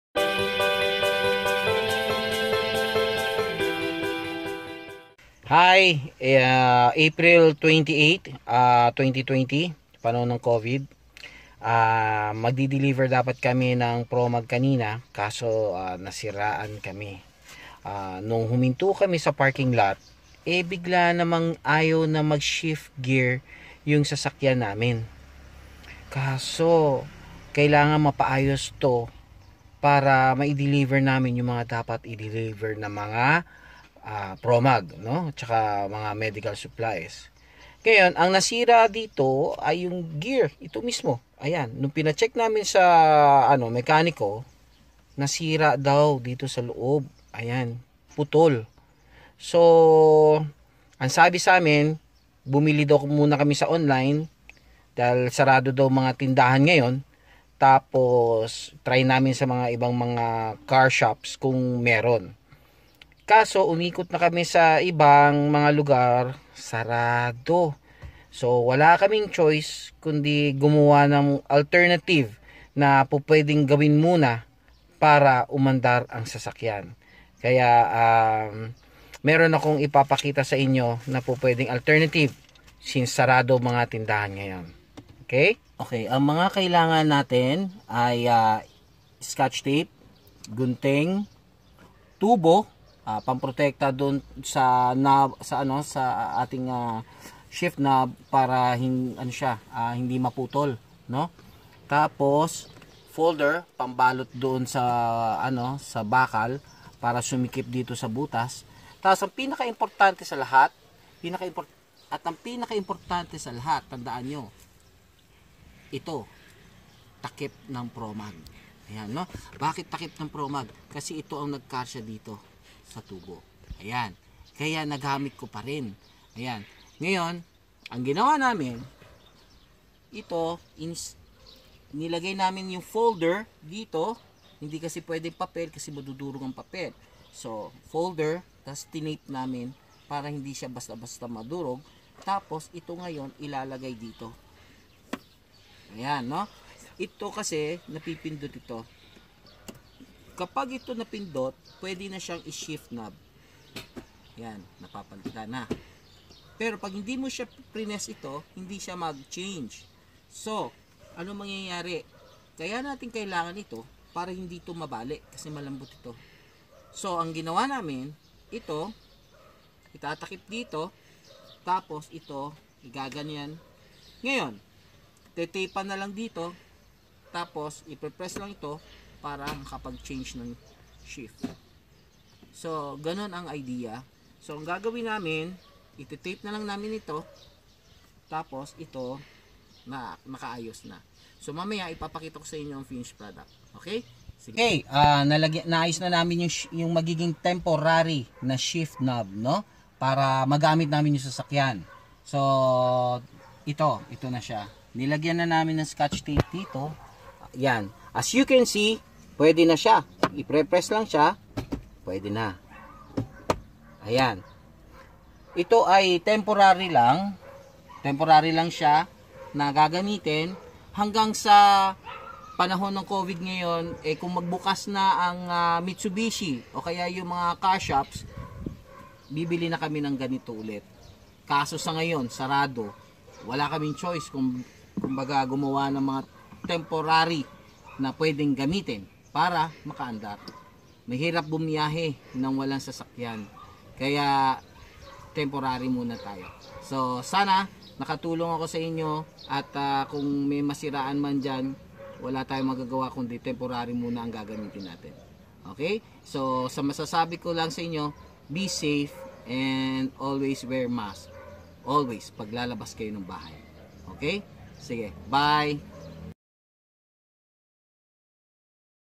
Temporary Shift Knob during ECQ
Nagamit ang Promag 300 bottle cap